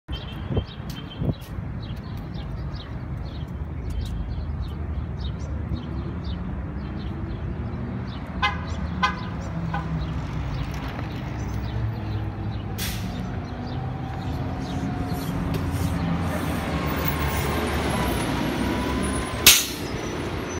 MTA New York City Bus: 2011 New Flyer C40LF CNG 256 B43 Arriving @Graham Ave & Scholes St
Filmed on Sunday, March 8, 2020 @6 PM.
Location: Graham Ave & Scholes St in Brooklyn, New York.

Flickr: NYC Transit Fanner Boy